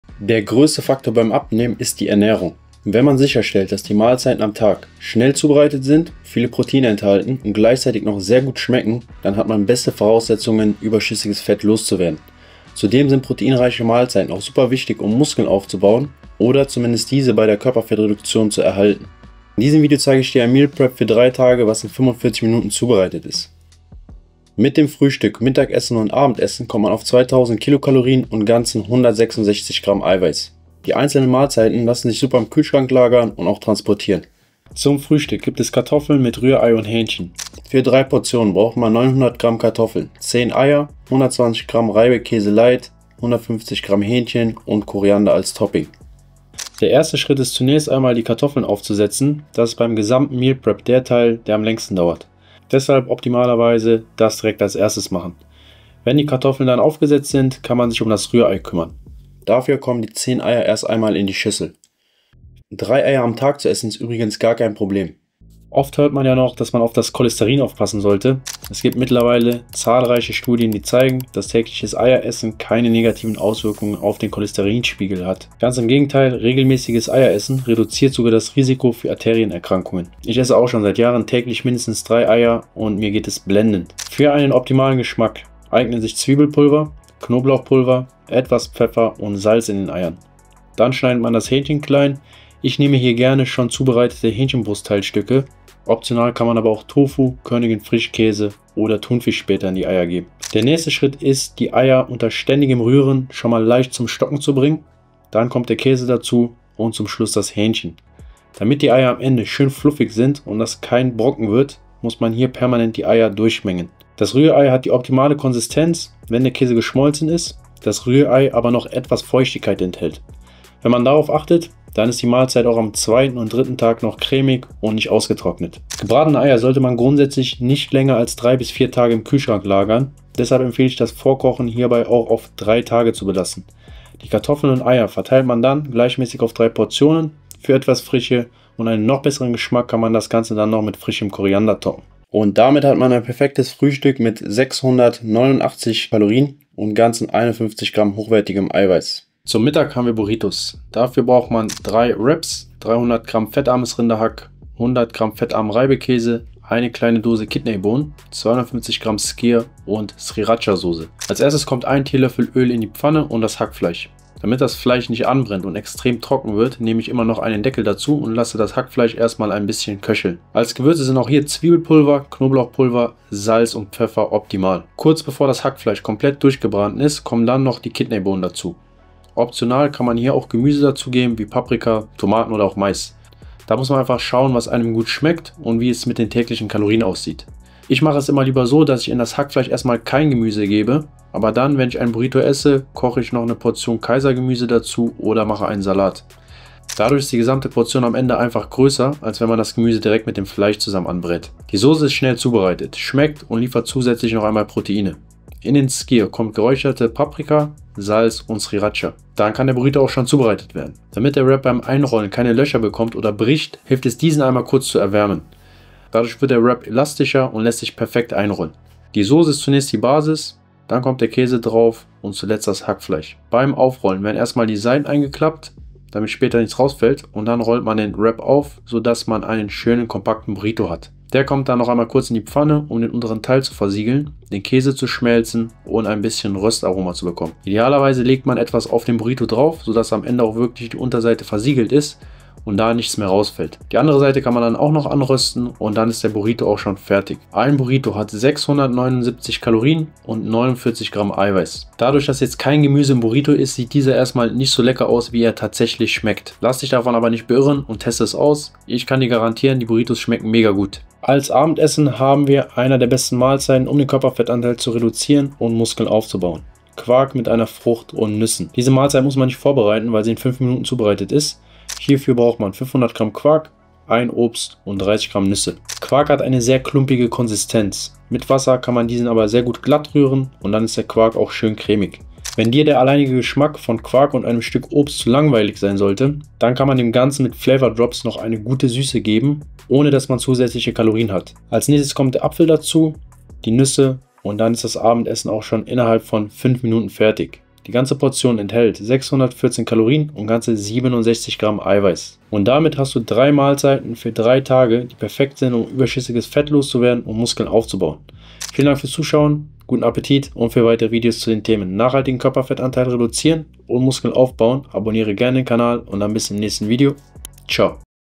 3 Tage VORKOCHEN und planbar ABNEHMEN | High Protein & Schnell!
Effizientes Vorkochen ist der Schlüssel zu nachhaltigem Fettabbau und Muskelaufbau. In diesem Video wird gezeigt, wie man in nur 45 Minuten Frühstück, Mittagessen und Abendessen für 3 Tage zubereiten kann – mit 2000 Kalorien und ganzen 166g Protein pro Tag. Die Mahlzeiten sind nicht nur proteinreich, sondern auch lecker und leicht vorzubereiten. Perfekt für alle, die ihre Ernährung optimieren und überschüssiges Fett loswerden möchten, ohne auf Genuss zu verzichten. Alle Rezepte lassen sich super lagern und einfach transportieren – ideal für einen stressfreien Alltag.

👥Du möchtest persönliche und individuelle Unterstützung bei deiner Körper-Transformation? Hier gehts zum Online-Coaching-Kontaktformular:

📩Newsletter: Bekomme regelmäßig praktische Tipps, um Fett zu verlieren und Muskeln aufzubauen

Hier sind einmal die einzelnen Lebensmittel der Mahlzeiten:
Kartoffel-Rührei-Hähnchen (3 Portionen):
900g Kartoffeln
120g Reibekäse fettreduziert
150g Hähnchen
10 Eier
Koriander

Burrito(3 Portionen)
3 Wraps
300g fettarmes Rinderhack
150g Kidney Bohnen
100g fettarmer Reibekäse
250g Skyr
Scharfe Soße

Quark mit Früchten und Nüssen (1 Portion)
500g Quark
1 Frucht
30g Nüsse
Die Glasboxen aus dem Video (5 Stück)

00:00-00:37 Übersicht
00:38-02:28 Frühstück
02:29-04:21 Mittagessen
04:22-05:18 Abendessen